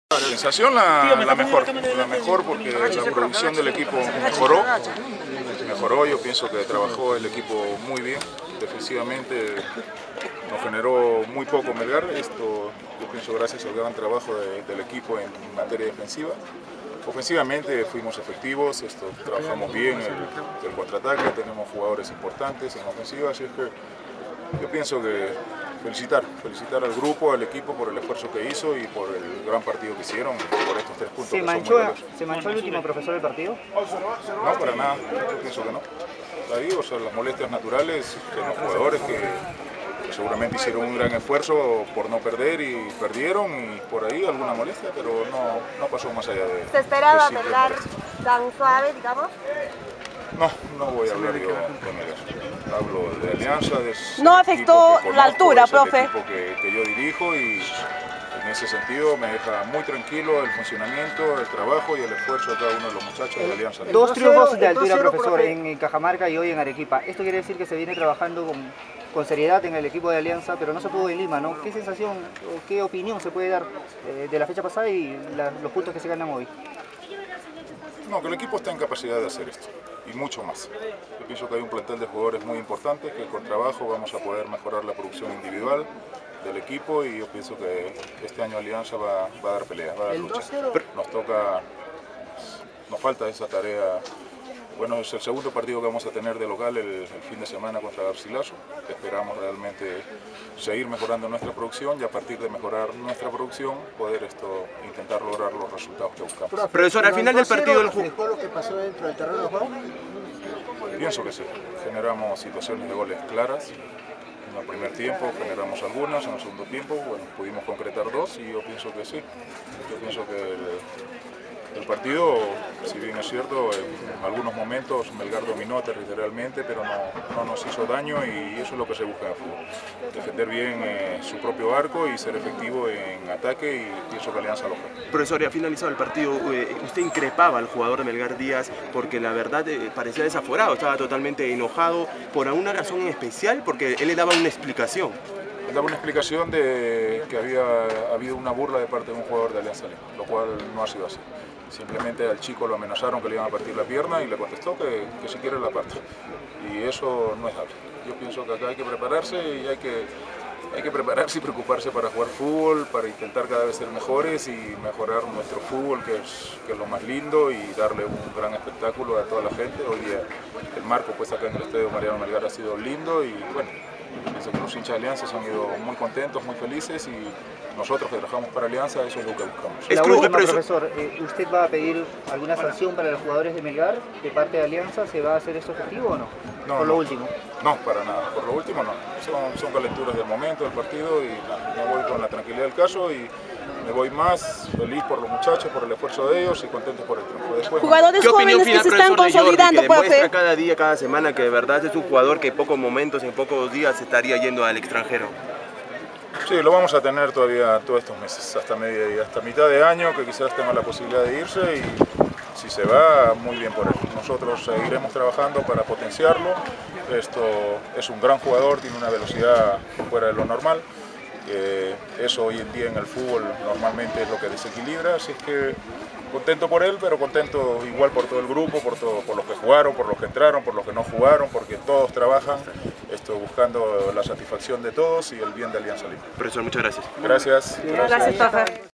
Palabras de Wilmar Valencia, Director Técnico de Alianza Lima
Para , la Web Deportiva del Sur del Perú, recogimos las palabras de Wilmar Valencia Director Técnico de Alianza Lima luego del triunfo del conjunto blanquiazul que venció a Melgar de Arequipa por 2 a 0, quedó satisfecho del desenvolvimiento de su equipo en su visita a Arequipa.

Para más información local, regional, nacional e internacional visite nuestra página web ... La Web Deportiva del Sur del Perú.